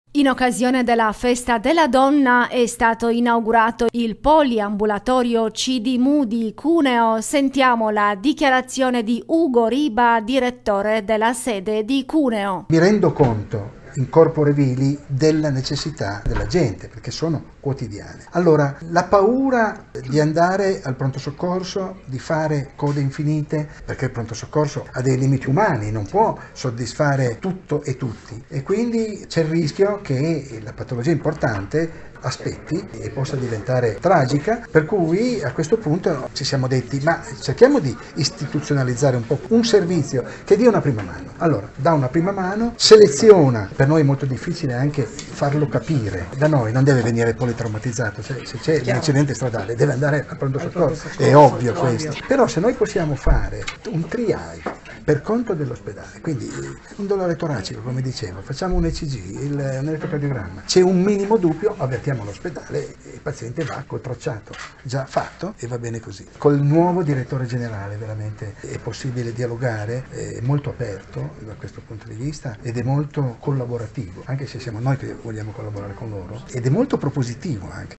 INAUGURAZIONE DEL NUOVO POLIAMBULATORIO CIDIMU A CUNEO | UGO RIBA
Ai nostri microfoni, Ugo Riba, Direttore di CIDIMU di Cuneo e fondatore dello stesso gruppo di Istituti.